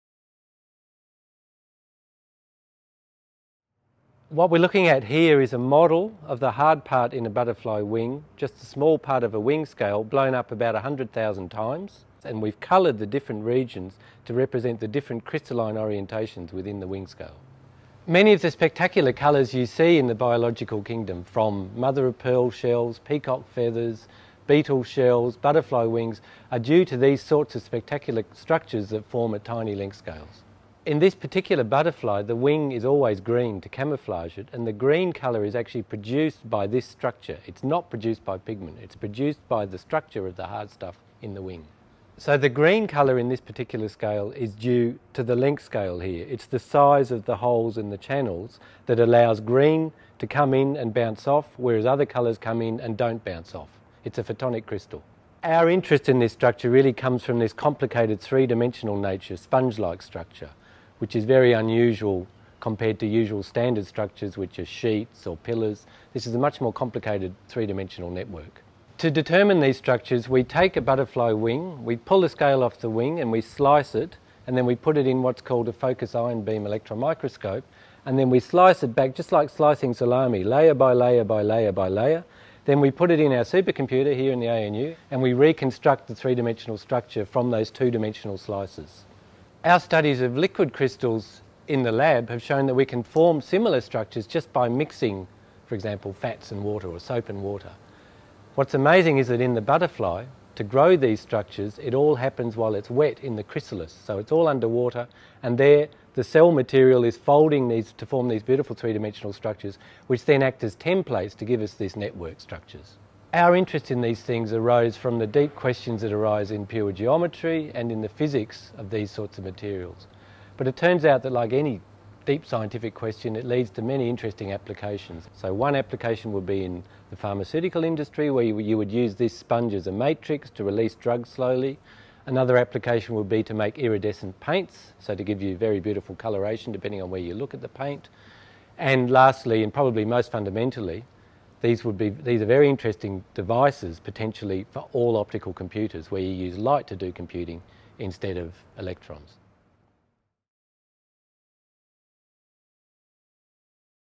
Flying colours
Who knew butterflies could be harbouring the blueprint for next-generation supercomputers or slow-release cancer drugs?

In this video Professor Stephen Hyde from the ANU Department of Applied Mathematics explains the secrets of photonic crystals, which give butterflies, beetles and peacock feathers their beautiful metallic colour.